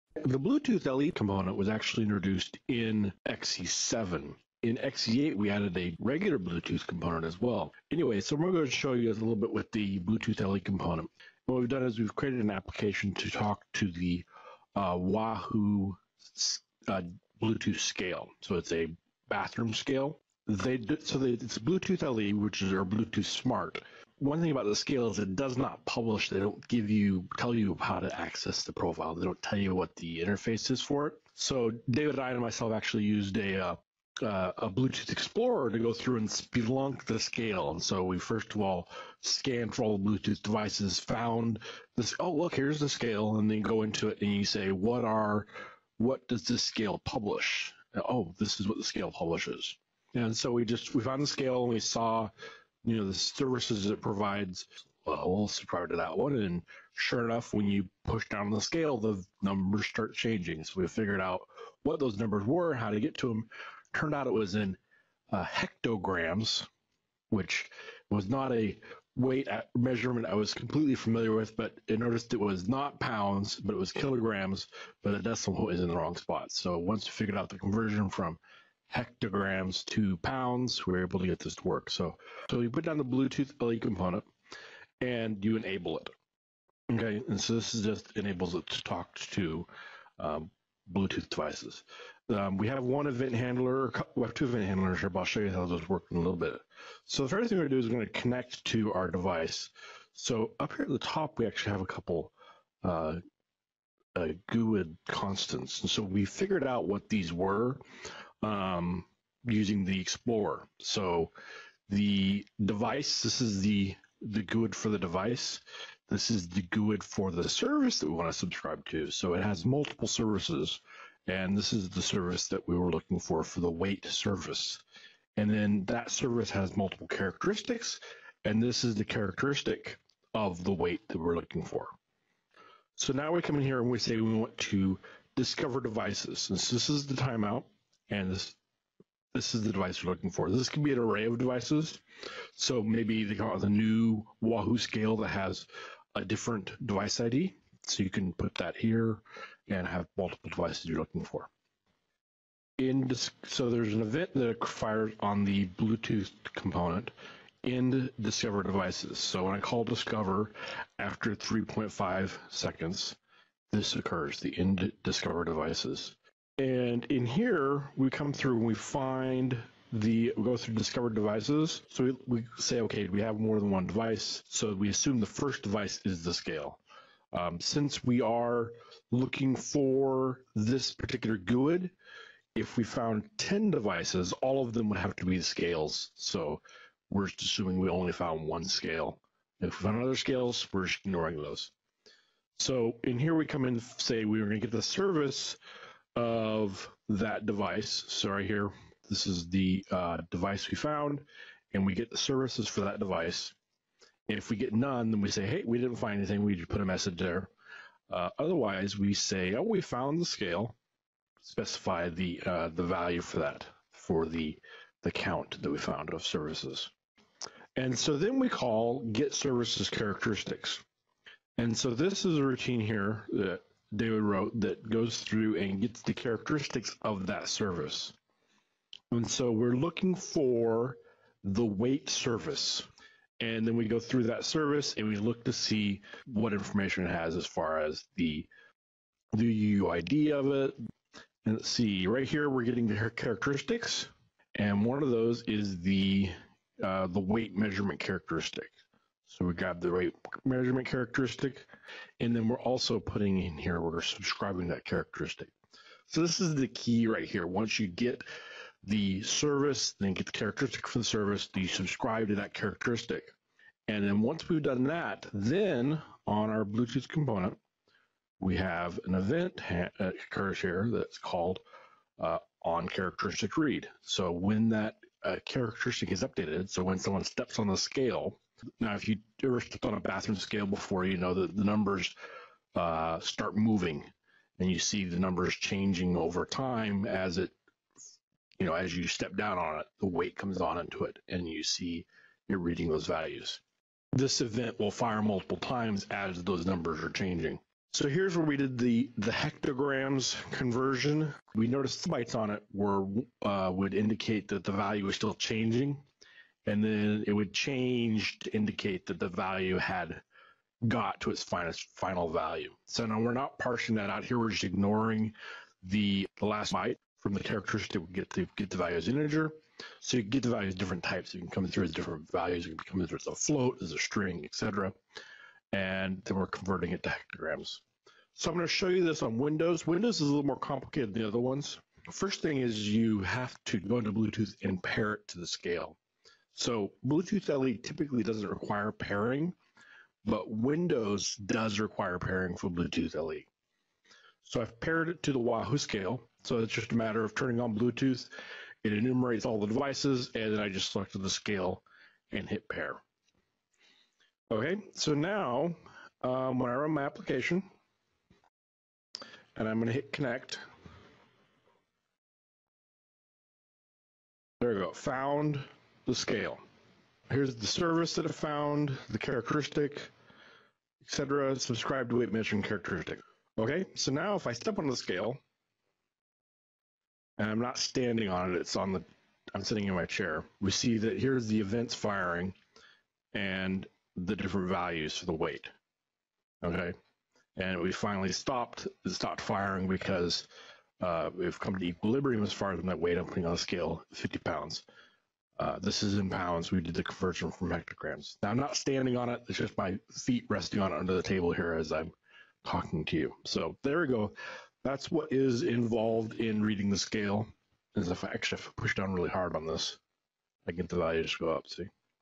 Working with Bluetooth LE and GATT Profiles
Using the Bluetooth LE component to manually discover and connect to a Bluetooth LE heart-rate monitor.